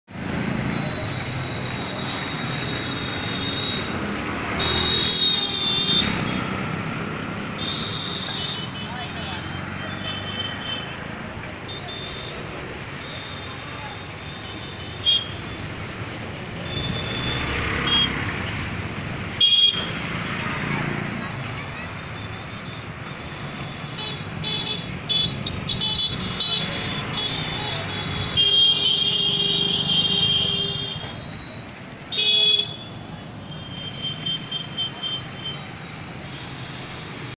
Tricycle Advertising || SmartAds.in || 9004001000.
Tricycle advertising is the best way to promote the brand at Locally.we are doing this activity at PAN India also. According to customer budget and requirements, we will customise it.
it will have two types which are as follows:-

1)Non-Lit Tricycle Advertising.
2) Back-Lit Tricycle Advertising.

👇For More Videos and Information regarding Advertising

About : Smart Ads is a YouTube Channel, where you will find All Advertising Related Videos And Information.